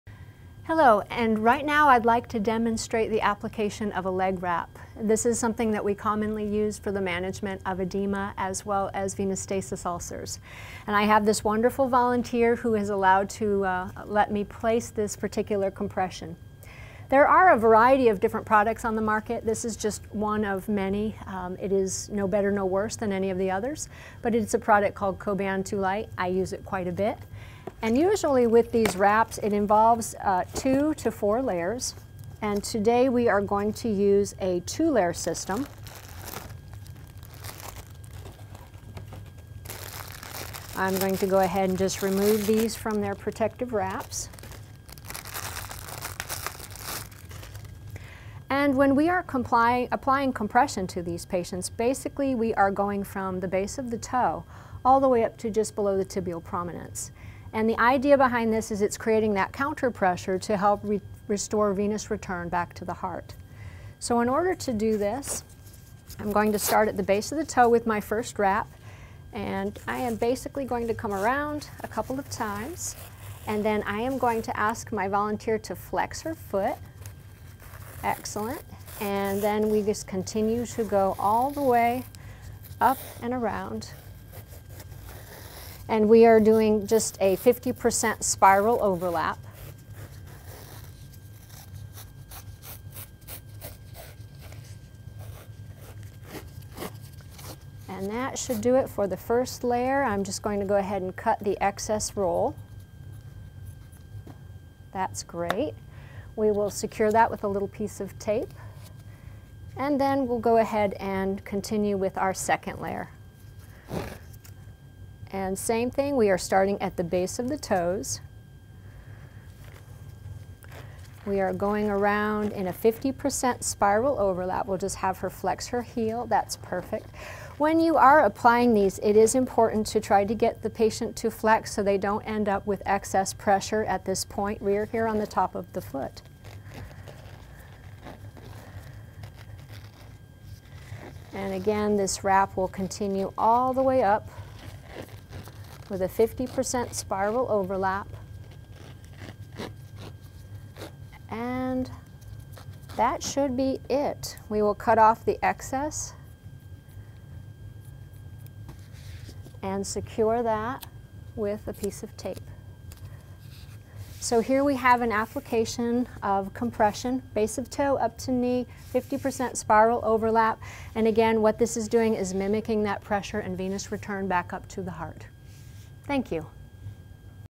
Leg Wrap / Compression
Leg wrap / compression is commonly used in the treatment of edema and venous stasis ulcers. Watch this step-by-step guide to properly applying a leg wrap / compression.

Wound Care Demonstrations
by Dianne Murray Rudolph, APRN, GNP-BC, CWOCN, UTHSCSA

We value your opinion - please let us know what you think of this video